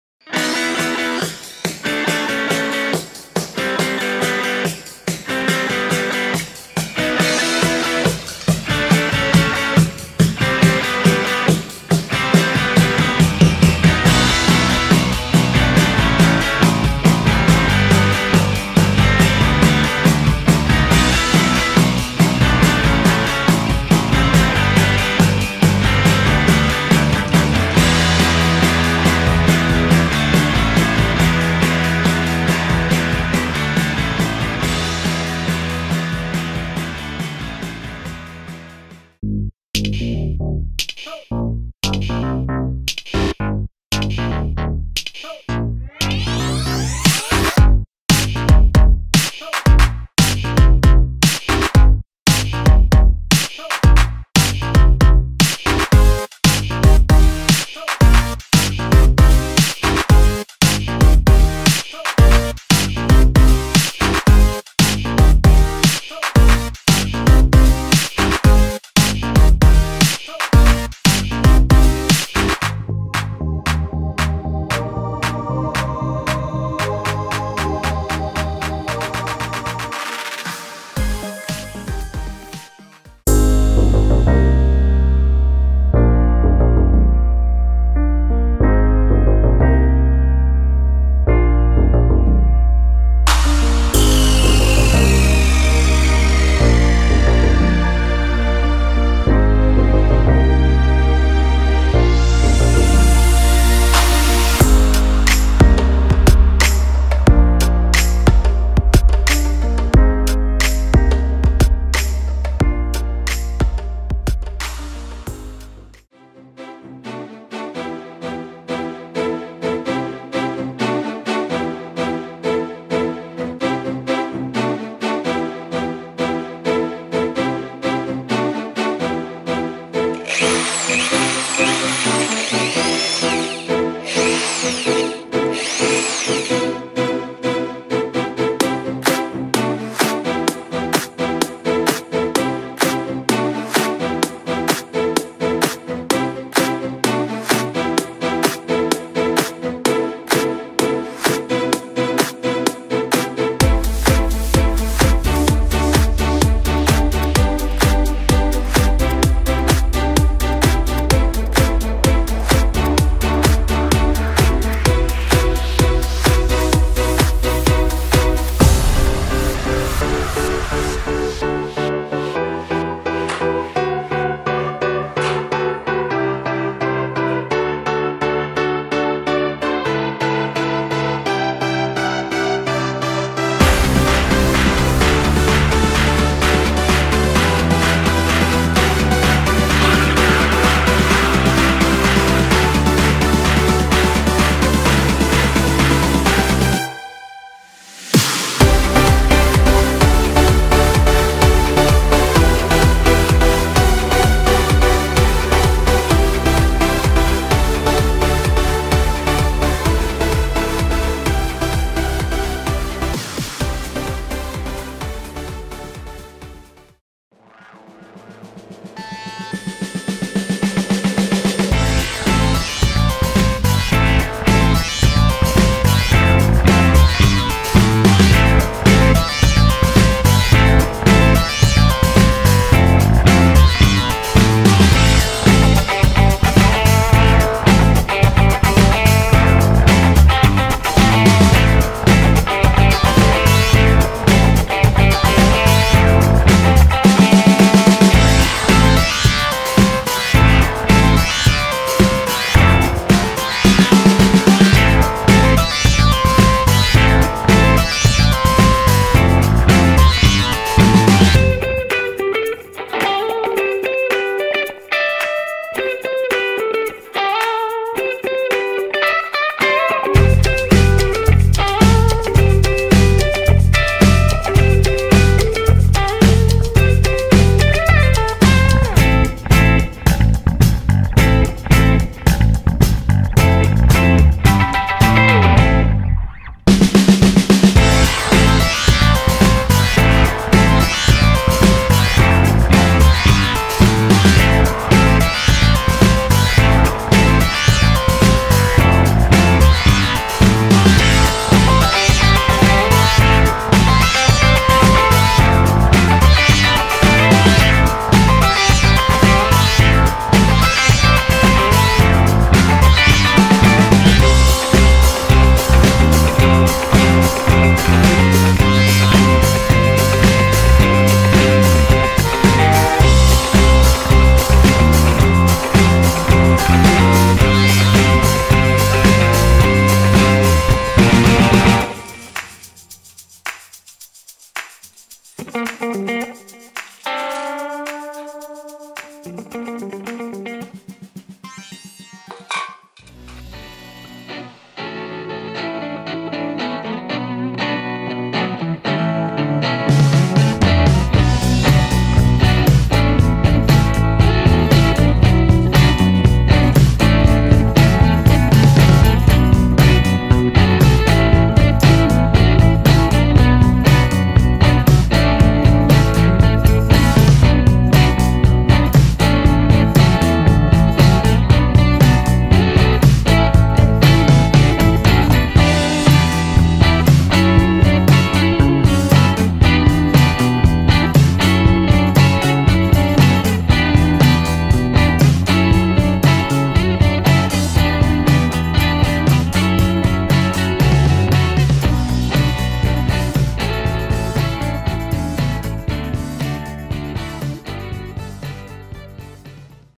10 trucchetti salvaeuro per la tua vita. I trucchi salvaportafoglio di Projectninna voto 10
Ciao Curioso e bentornato ad un nostro nuovo video.
Anche stavolta proveremo a rendere la tua vita più semplice con i nostri Epici life hacks, tutorial... i nostri trucchi salvaportafoglio insomma.
Se hai domande o per motivi commerciali vuoi contattarci non esitare a farlo!